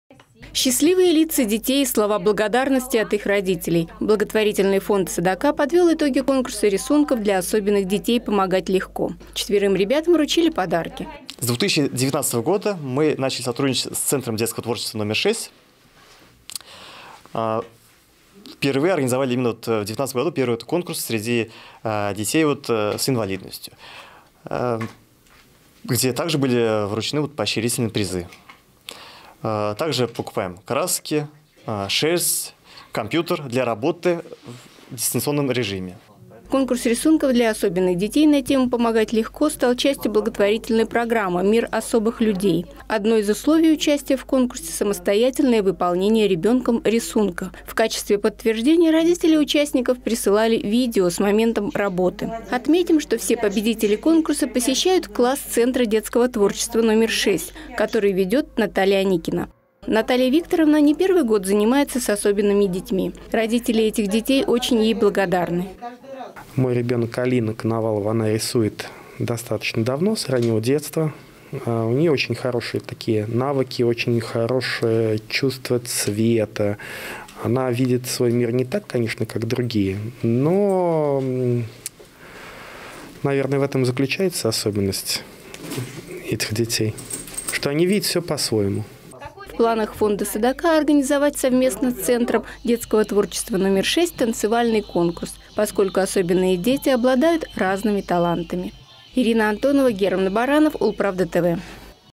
Помогать легко. Что рисуют особенные дети
Величественные горы, осенний лес и красивые цветы. Счастливые лица детей и слова благодарности от их родителей. Благотворительный фонд «Садака» подвёл итоги конкурса рисунков для особенных детей «Помогать легко».